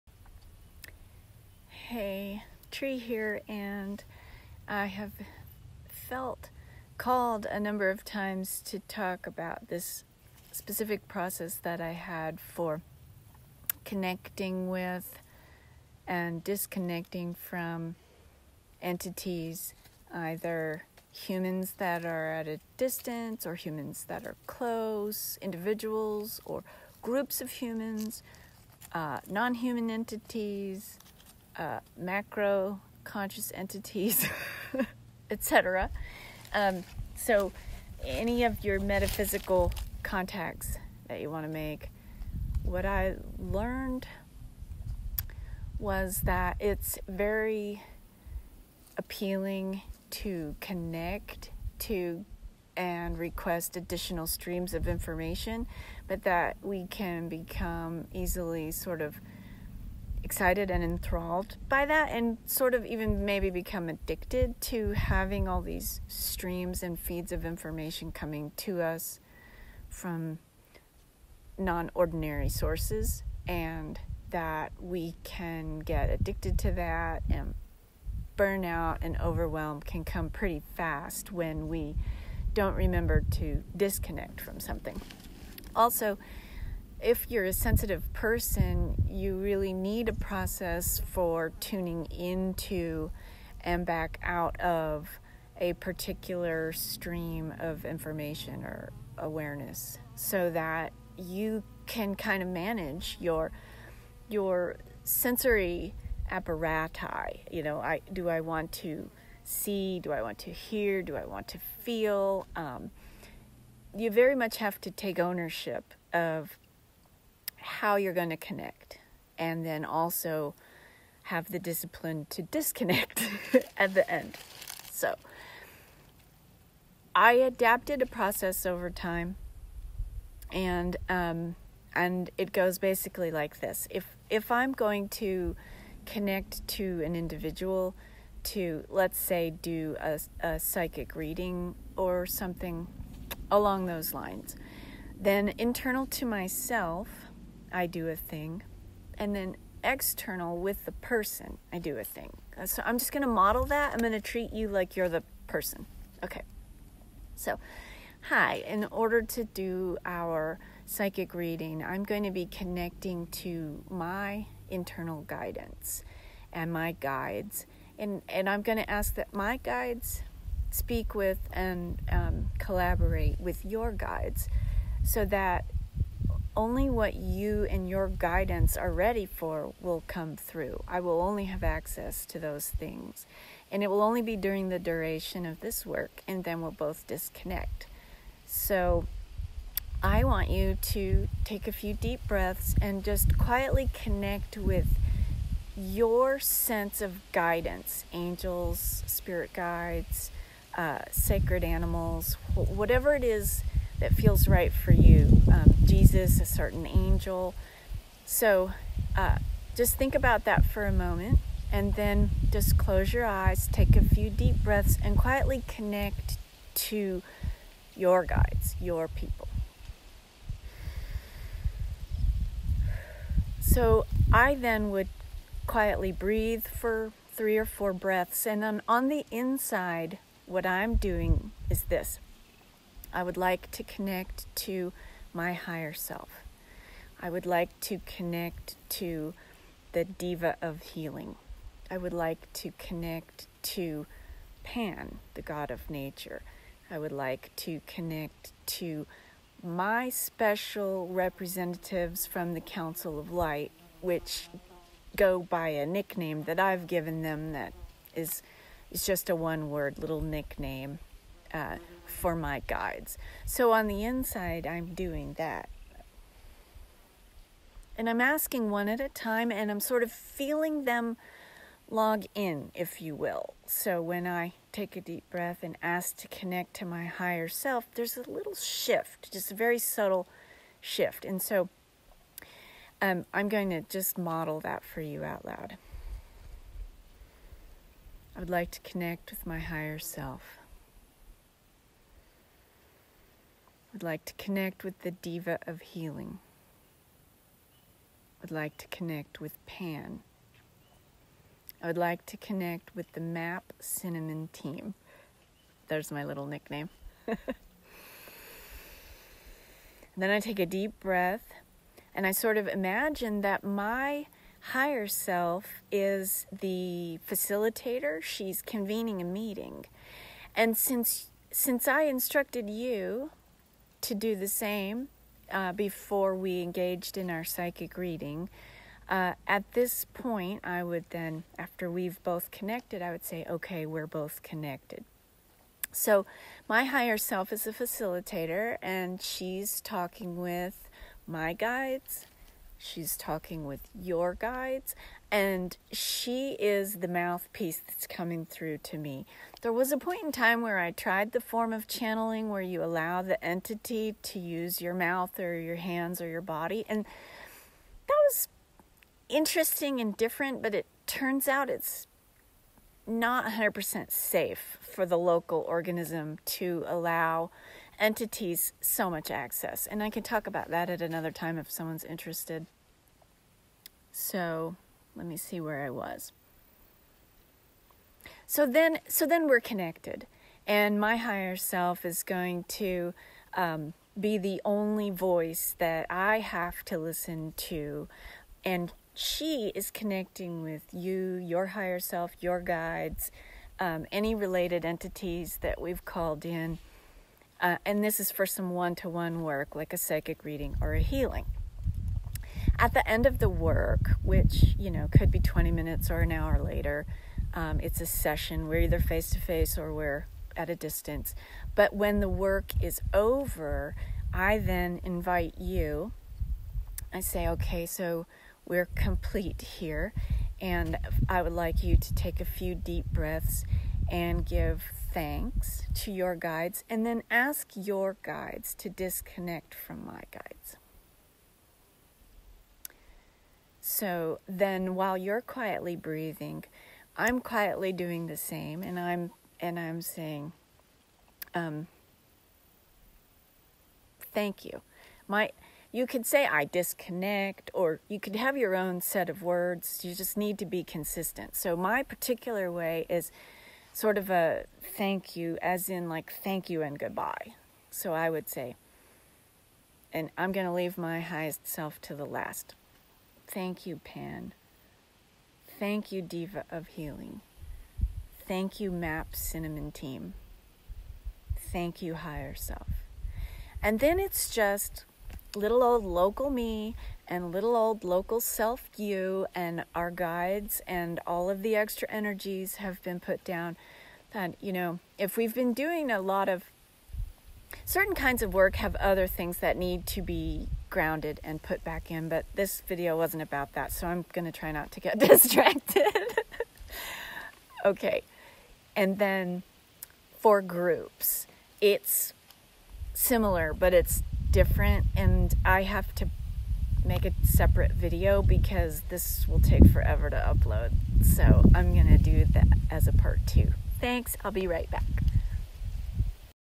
Connecting and Disconnecting Higher Sense Perception, Part 1
When using higher sense perception it is important to have a consistent process for connecting to and disconnecting from the entities. Part one looks at a one to one connection between two people during a healing or a psychic reading.